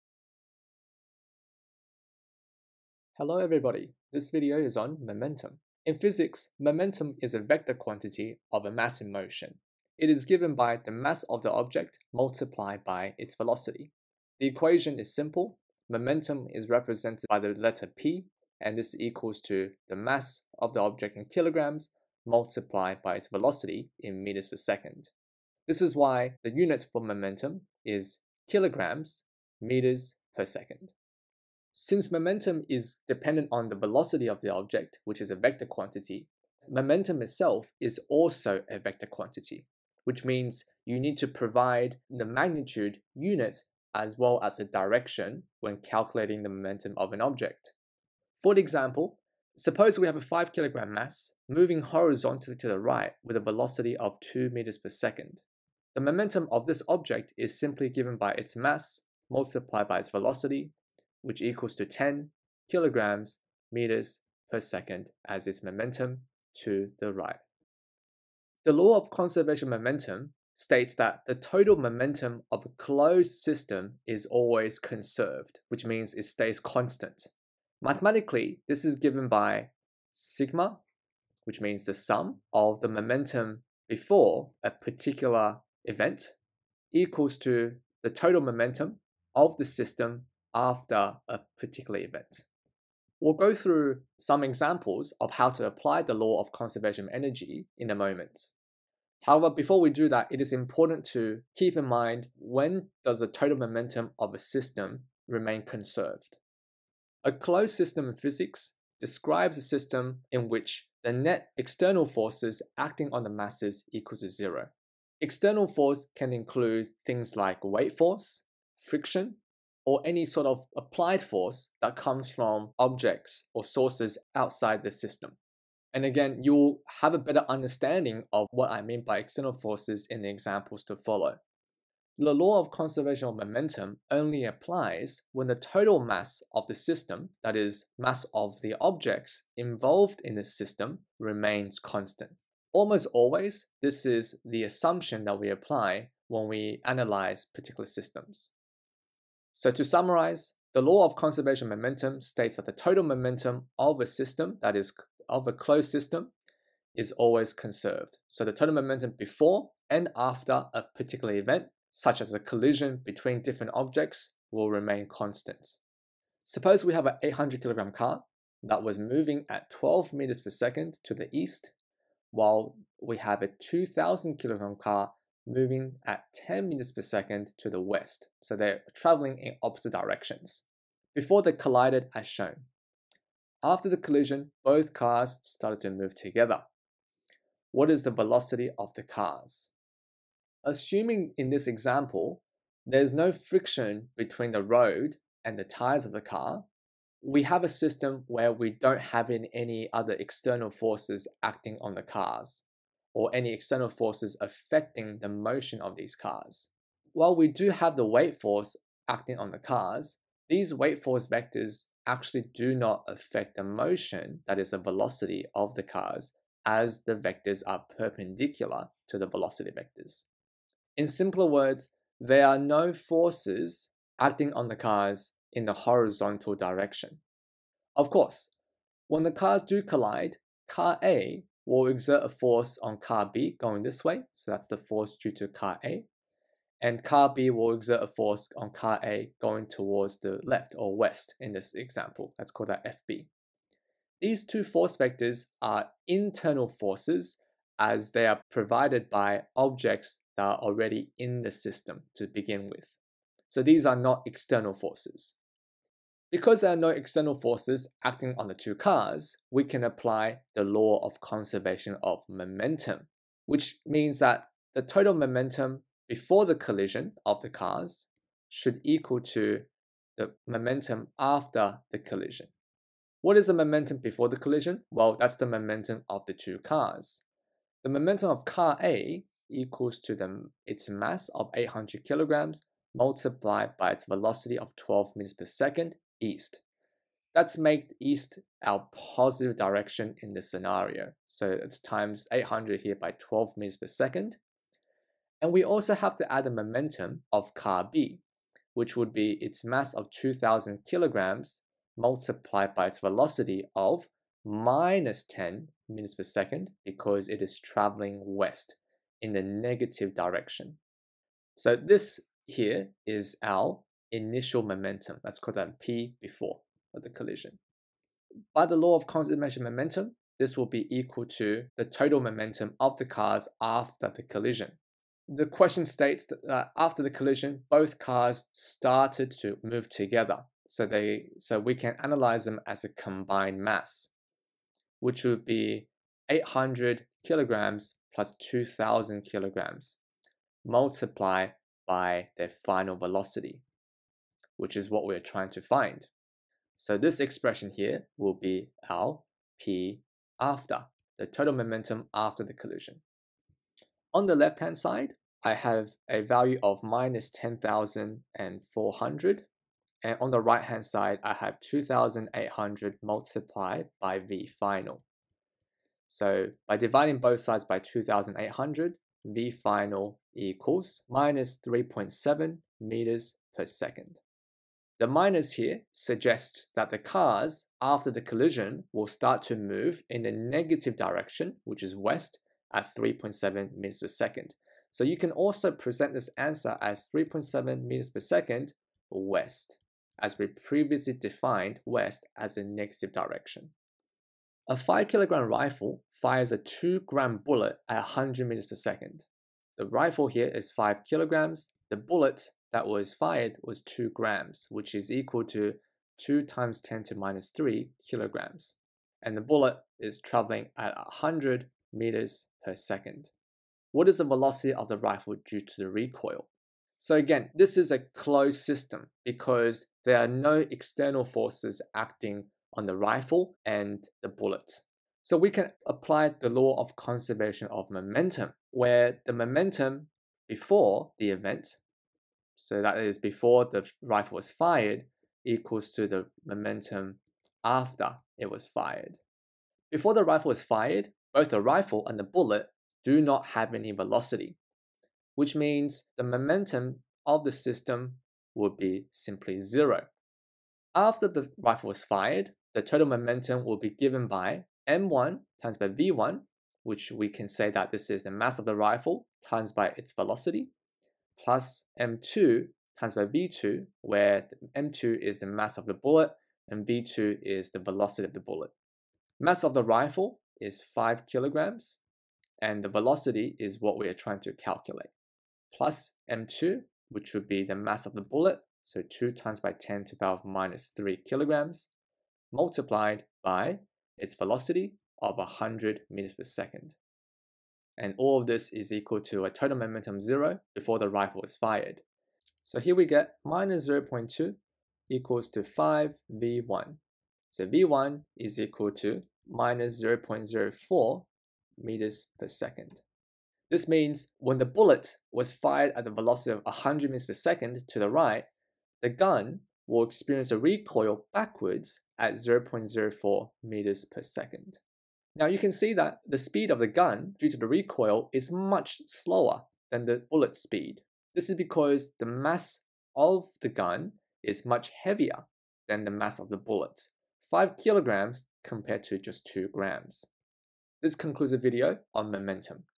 Momentum & Law of Conservation of Momentum // HSC Physics
⌚Timestamp
00:00 What is momentum?
01:08 Law of Conservation of Momentum
02:46 Example 1 – Collision between two masses
06:34 Example 2 – When initial total momentum is zero

📚Syllabus
• conduct an investigation to describe and analyse one-dimensional (collinear) and two-dimensional interactions of objects in simple closed systems (ACSPH064)
• analyse quantitatively and predict, using the law of conservation of momentum Σ𝑚𝑣before= Σ𝑚𝑣after and, where appropriate, conservation of kinetic energy Σ1/2𝑚𝑣before= Σ 1/2  𝑚𝑣after, the results of interactions in elastic collisions (ACSPH066)